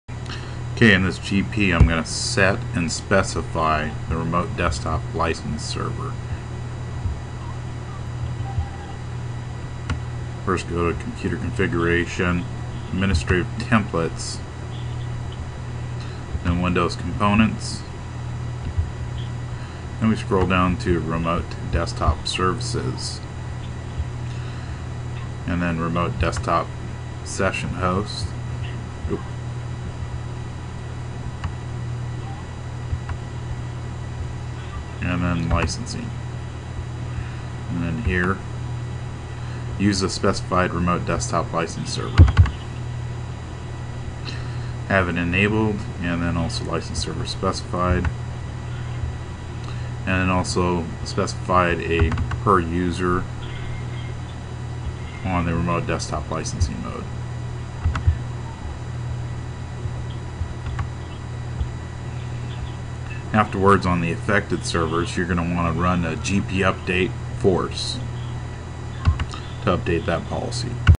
Citrix XenApp 6.5 Setting RDS license server with a GPO
After setting the RDS licensing server run a gpupdate /force on all the servers in the OU that is linked to your policy.